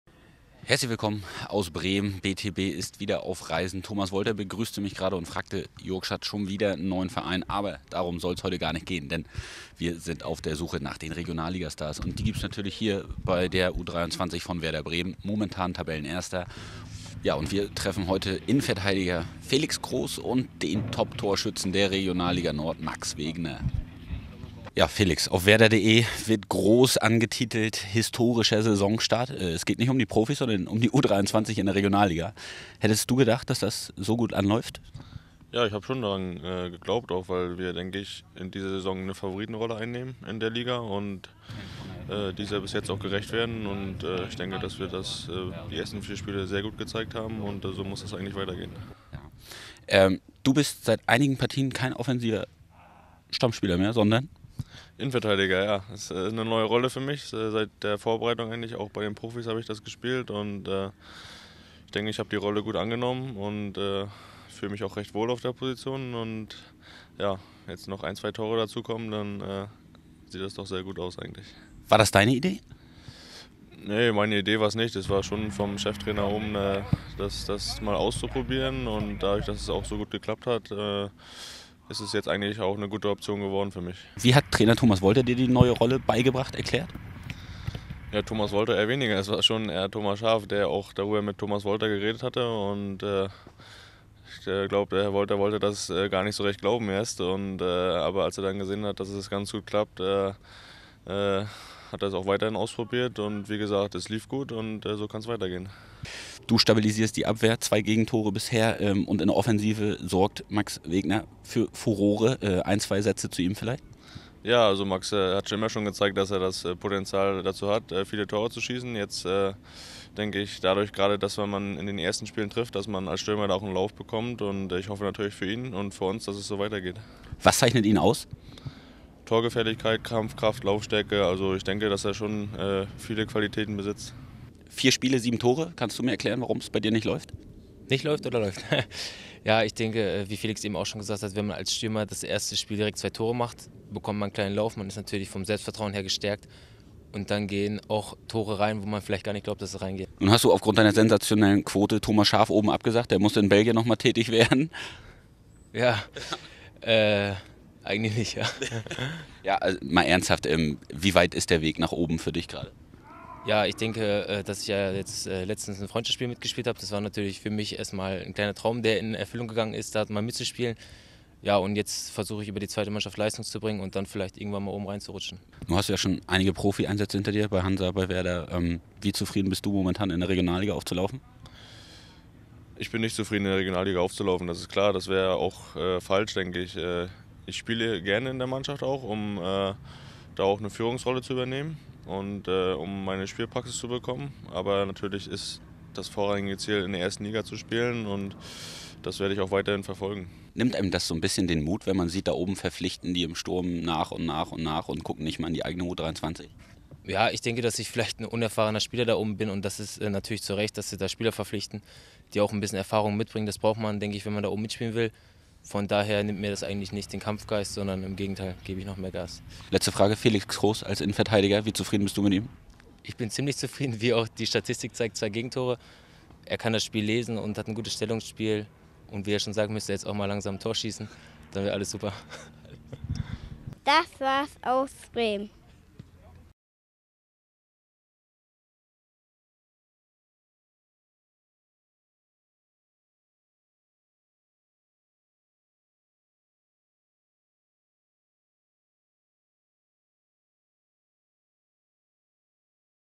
Felix Kroos und Max Wegner im BTB-Interview | BLOG-TRIFFT-BALL
Das U23-Team von Werder Bremen mischt die Regionalliga Nord übel auf. BLOG-TRIFFT-BALL traf zwei Grün-Weiße: Felix Kroos und Max Wegner.